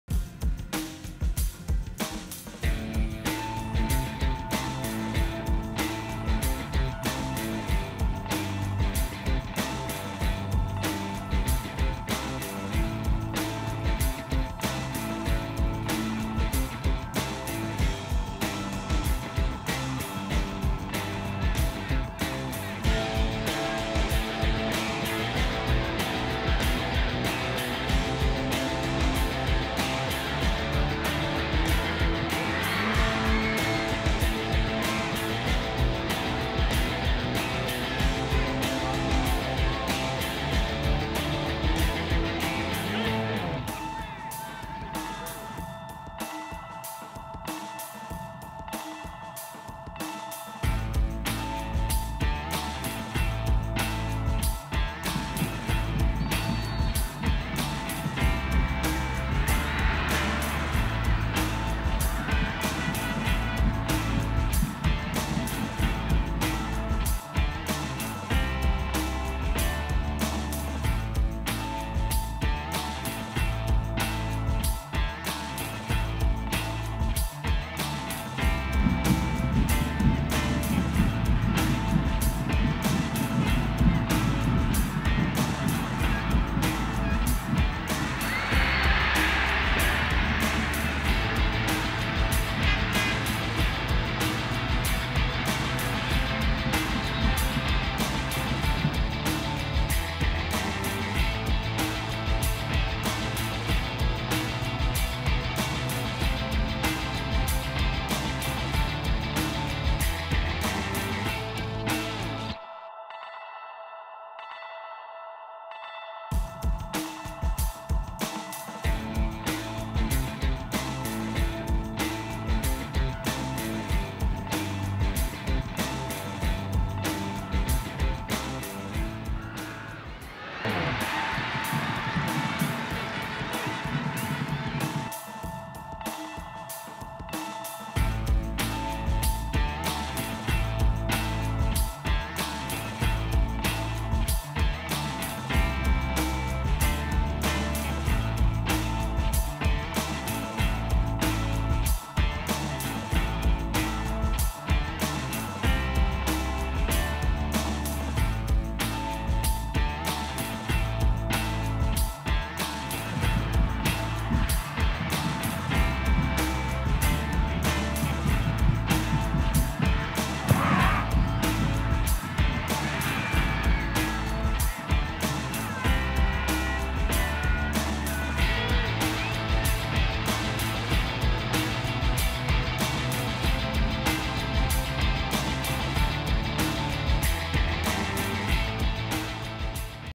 "SUPER SAVE" A.K AHMAD KURNIAWAN HALAU SERANGAN PREDATOR PERSITARA! ISL 2009/2010
Youtube: @VIA Entertainment , @PesbukersTV , @Kangenjoget , @VIA Misteri
Helo Apps: Lensa Olahraga | Pesbukers TV